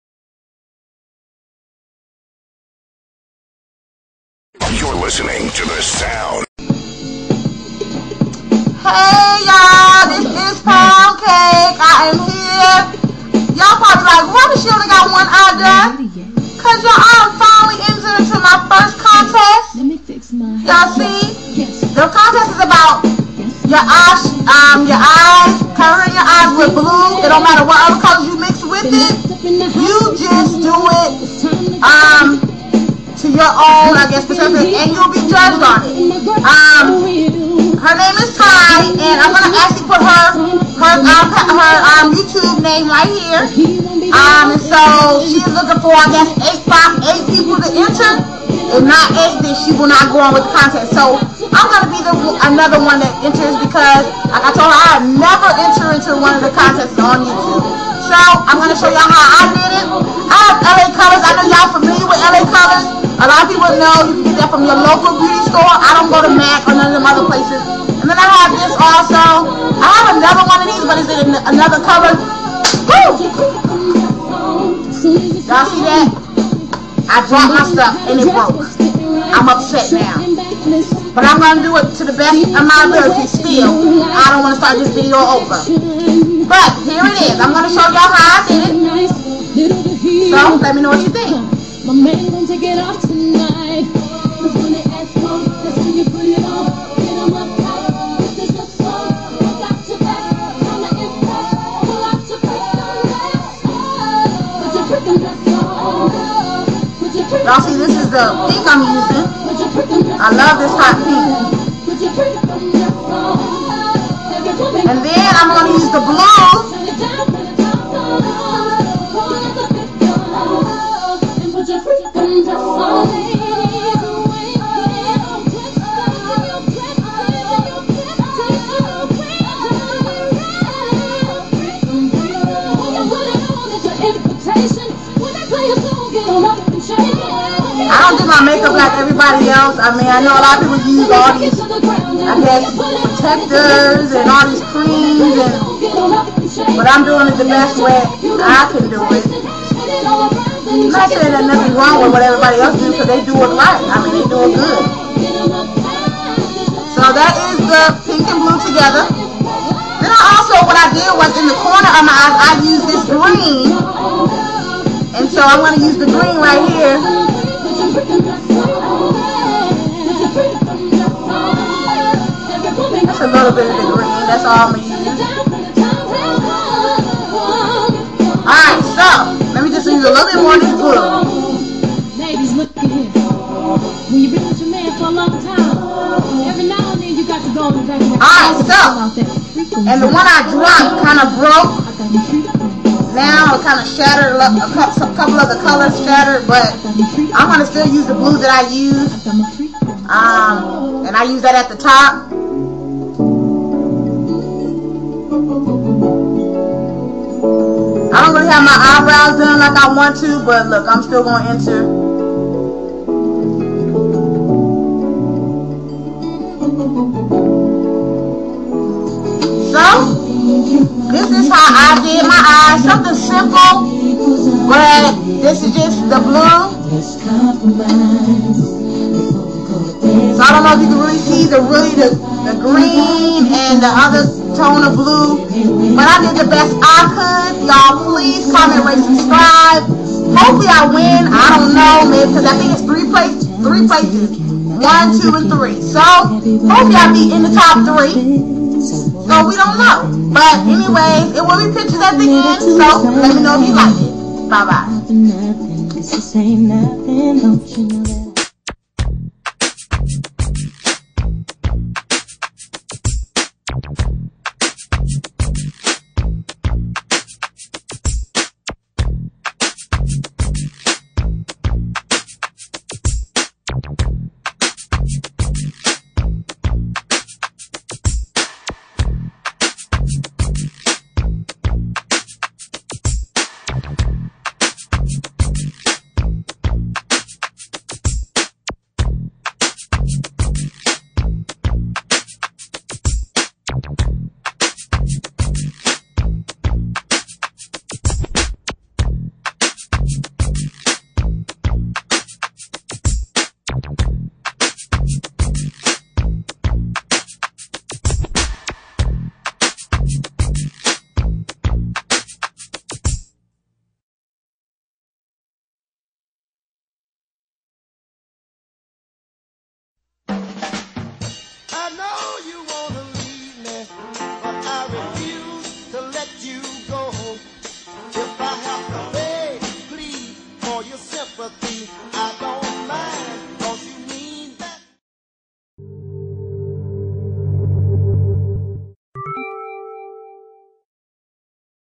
My First Contest Entry for Mstyana
Make it Rain with the Blue eyeshadow that means do your eyes in blue & You can also add any other color,Have fun!